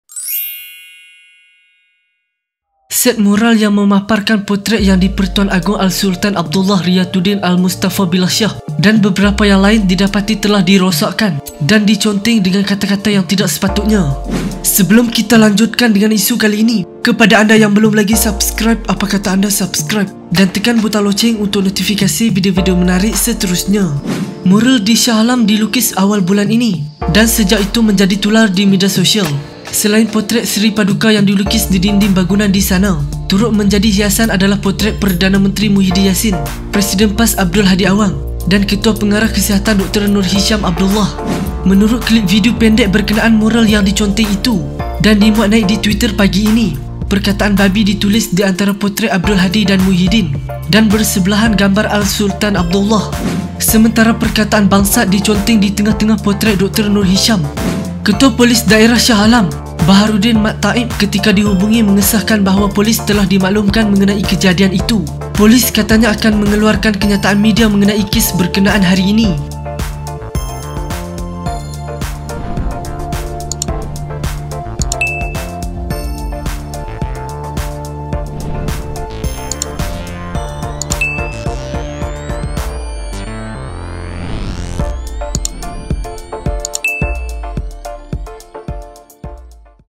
TERGAMAKNYA! Mural Dr Noor Hisham, Agong & Pemimpin Negara Dirosakkan Dengan Kata-Kata Kesat!
Selain potret Seri Paduka, yang dilukis di dinding bangunan di sana, turut menjadi hiasan adalah potret Perdana Menteri Muhyiddin Yassin, Presiden PAS Abdul Hadi Awang dan Ketua Pengarah Kesihatan Dr Noor Hisham Abdullah.